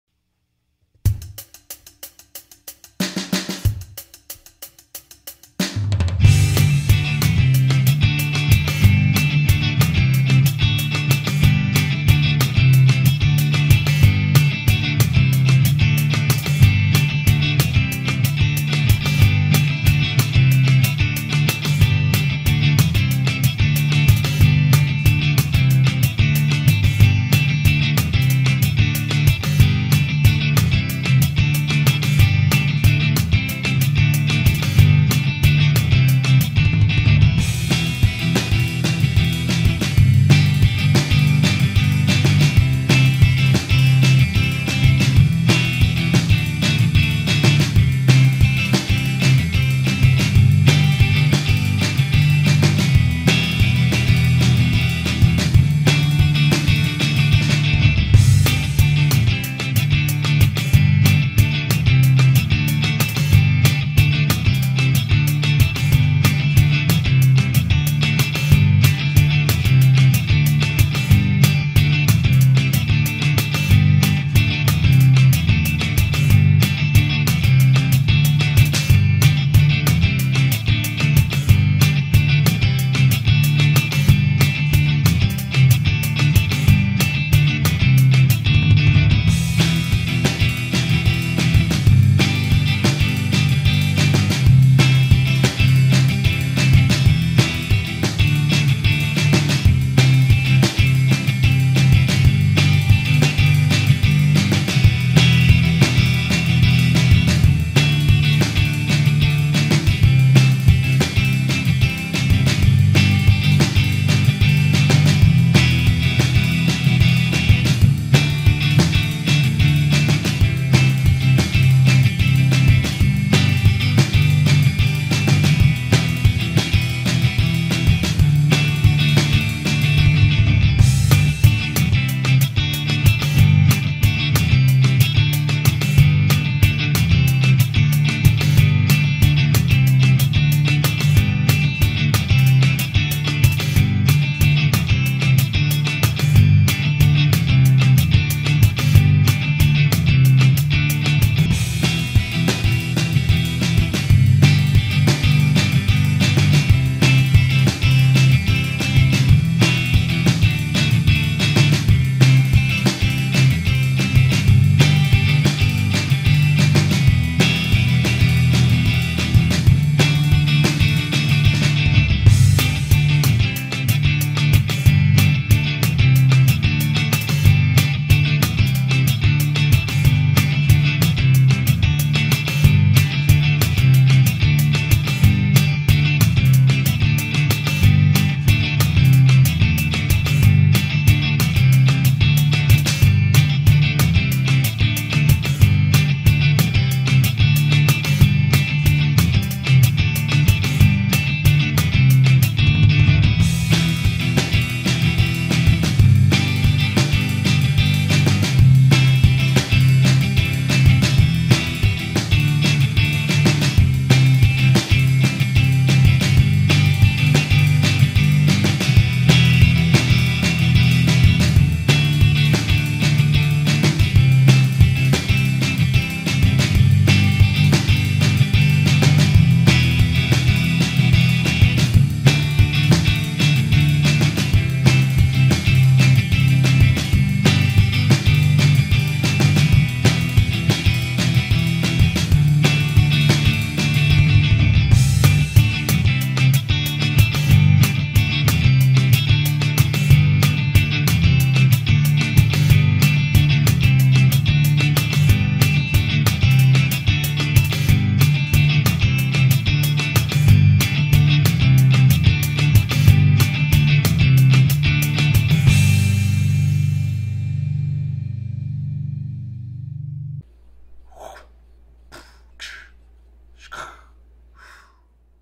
Upbeat Jam Track in G major with sliding chord numbers for CAGED system guitar soloing practice.
First I play the chords in as many different inversions as I can all over the fret board. Then I do the same with arpeggios.
After that I will play the G major scale and try to find some chord tones that aren't in G major.
Finally I will play the G major pentatonic scale and try to throw in arpeggios and major scale runs while keeping the current backing chord in mind.